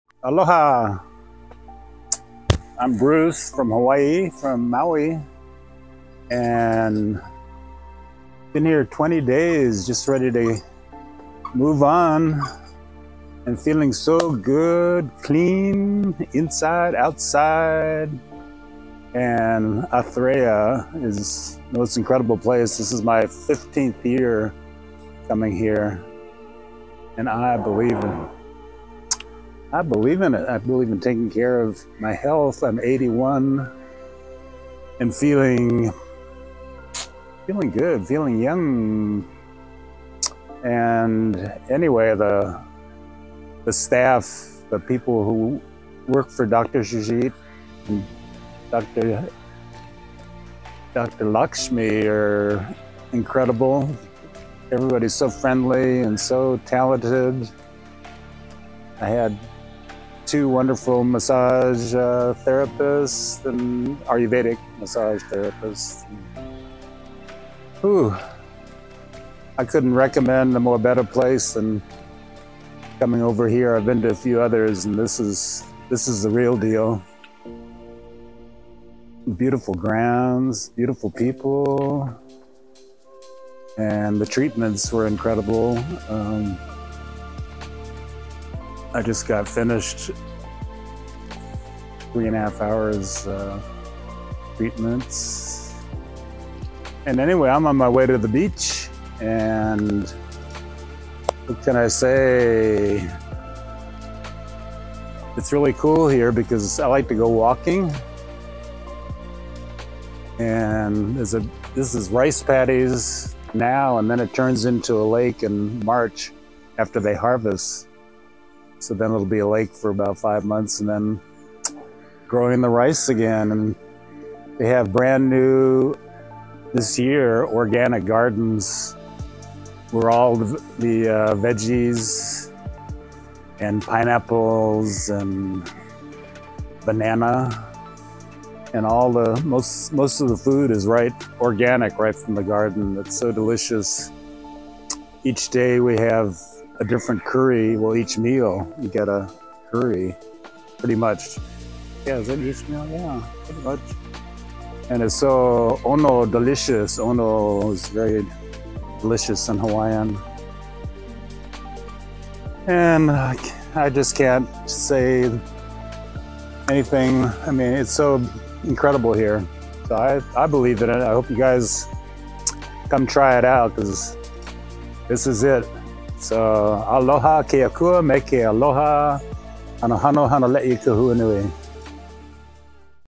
🌴 Aloha from Alfred Bruce! 🌟 - Athreya Ayurvedic Centre
🌴 Aloha from Alfred Bruce! 🌟
After 15 years of visiting Athreya, Alfred from Maui, Hawaii, shares his incredible journey of rejuvenation, health, and happiness. 🧘‍♂️💚

💬 "I’m 81, feeling young, and Athreya is the most incredible place. The treatments, the organic meals, the beautiful grounds—it’s all real and life-changing. I believe in it, and I hope you come try it out too!"

💆‍♂️ Experience personalized Ayurvedic care, warm hospitality, and the serenity of nature—all at Athreya Ayurvedic Centre. 🌿